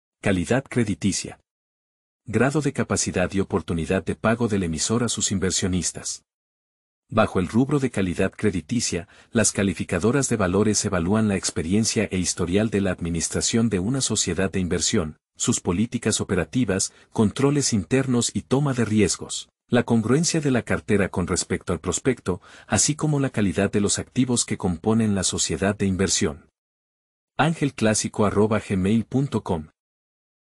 CALIDAD CREDITICIA
Grado de capacidad y oportunidad de pago del emisor a sus inversionistas. Bajo el rubro de calidad crediticia, las calificadoras de valores evalan la experiencia e historial de la administración de una sociedad de inversión; sus políticas operativas, controles internos y toma de riesgos; la congruencia de la cartera con respecto al prospecto, así como la calidad de los activos que componen la sociedad de inversión.